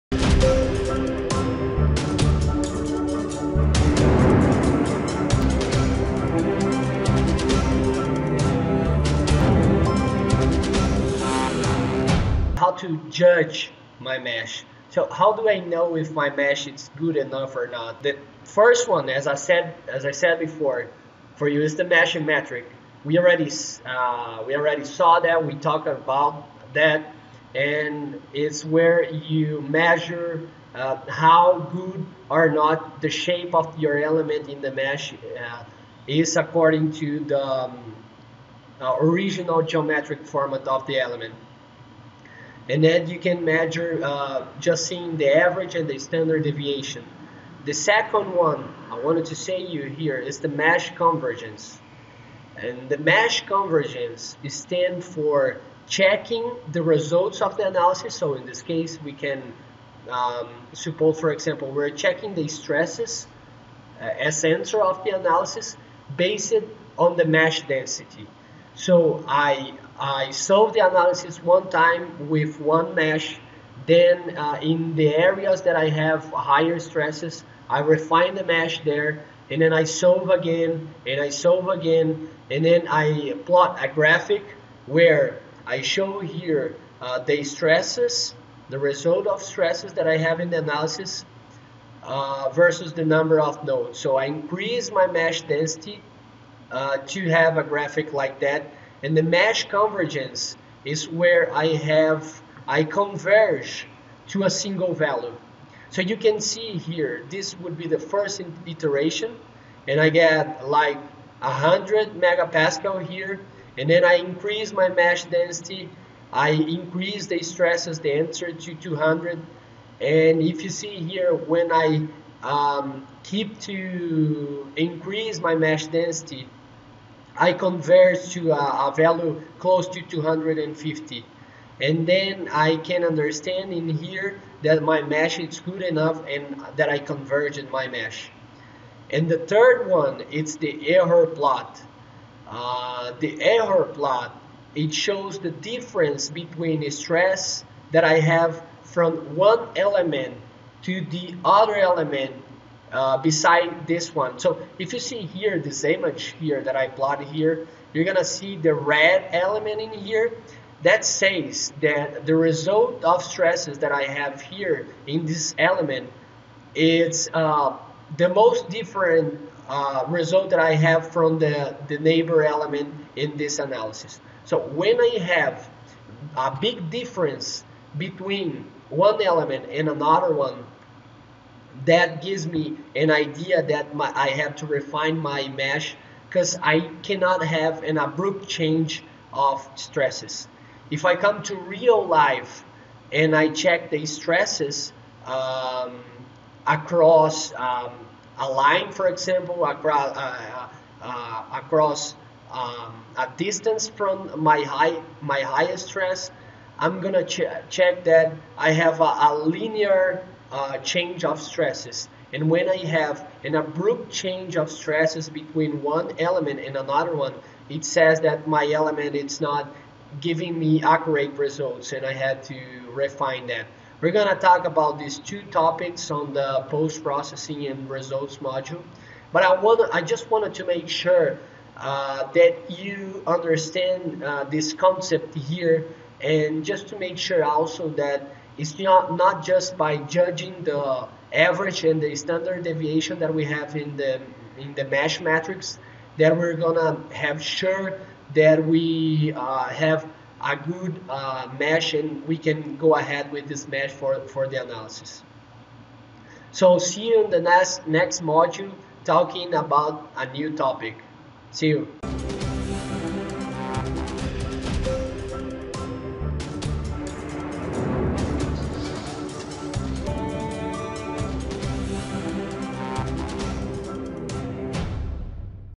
HOW TO JUDGE MY MESH??
In this video, it is shown 03 approaches to evaluate and judge the mesh generated by Ansys Workbench.